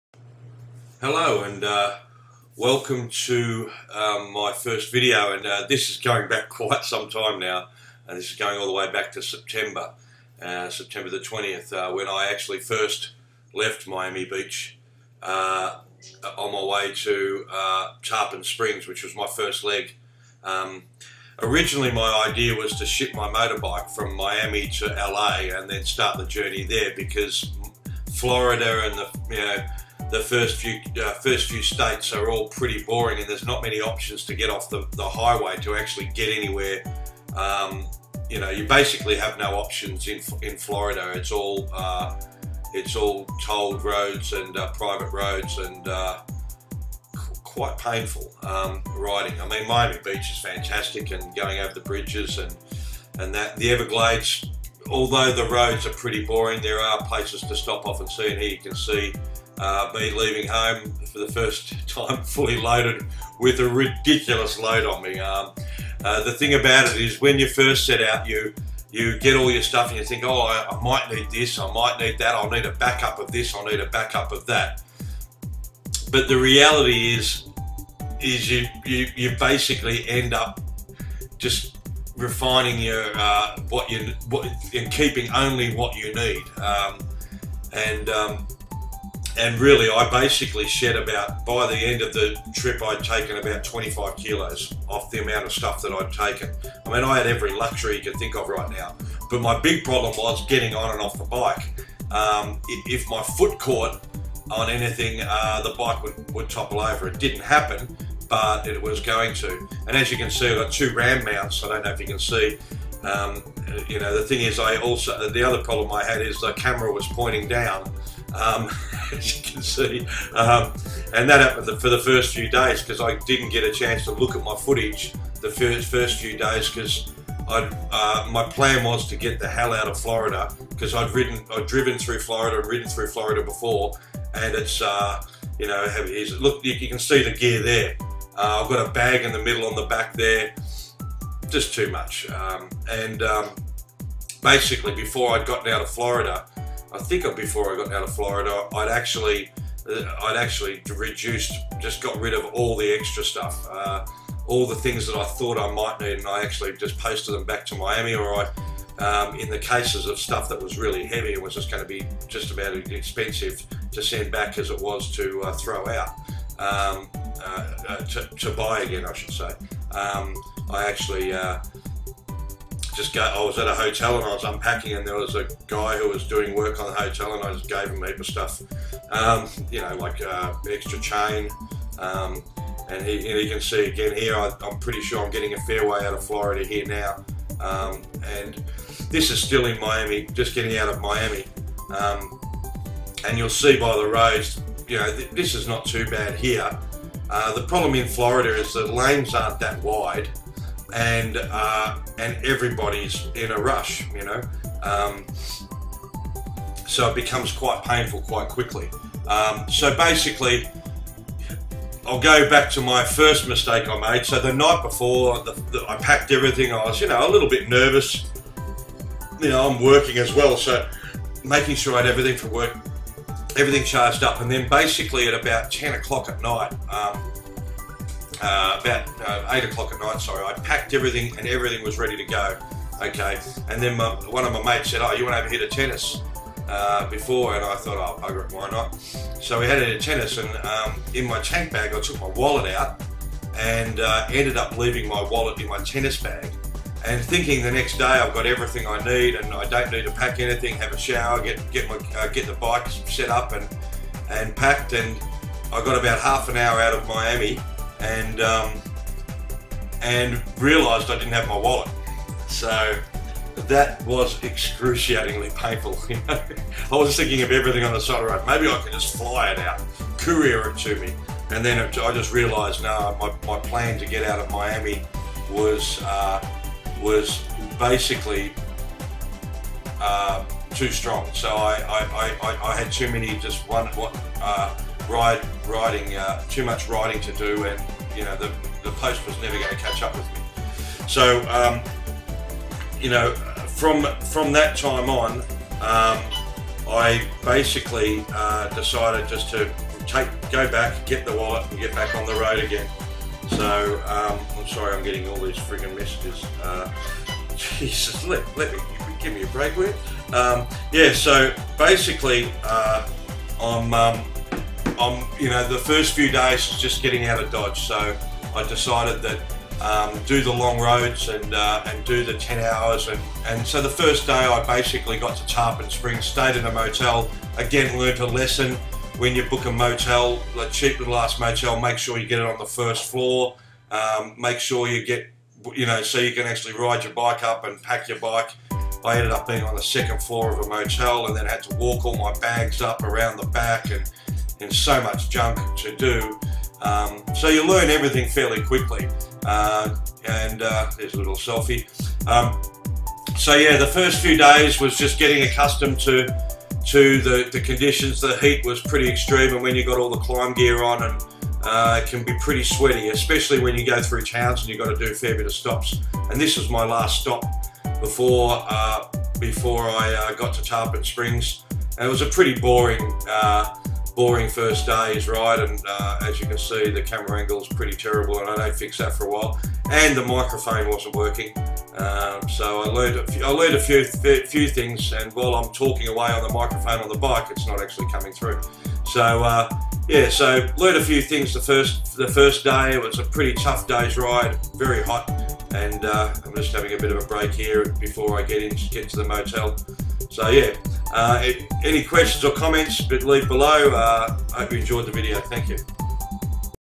Ride Day 1: Miami Beach to Tarpon Springs, Florida
After getting off to a rocky start leaving my wallet at home and only realizing this - 30 minutes into the journey, my days 272-mile (472 kilometres) journey began in Miami Beach heading north to Hollywood and Fort Lauderdale. The roads out of Miami Beach for at least the first hour were pretty terrible (traffic, idiots) but once I got out of the city limits I was well on my way.

It was a pretty hot day and with my Klim Overlander clothing, I was sweating pretty badly. I took a few stops along the way to get some water onboard and once I got into the everglades a nice breeze accompanied me for the rest of the journey.

The trip took me through Big Cypress National Preserve, Fakahatchee Strand State Preserve, Bonita Springs, Caloosahatchee Creeks Preserve, Port Charlotte, St Petersburg and finally into Tarpon Springs.

One thing I realized was that I could not wait to get out of Florida, flat straight roads, tight lanes and hot as hell and no real alternatives.

One other thing was becoming very clear to me on my way to Tarpon Springs. That I had too much gear. My KTM 1290 Super ADV could easily carry the load, however getting on and off the bike was a real issue and at times my foot was getting caught on its way across the seat and tipping the back was going to happen sooner rather than later.